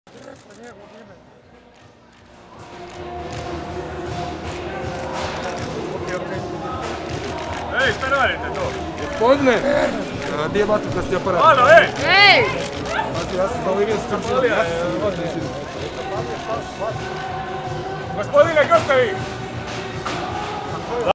Muškarac u stožeru Možemo! vrijeđao okupljene i zapalio ulaz
Nezadovoljni građanin vrijeđao je okupljene u Muzeju suvremene umjetnosti, polio benzinom stepenice i zapalio te je otišao.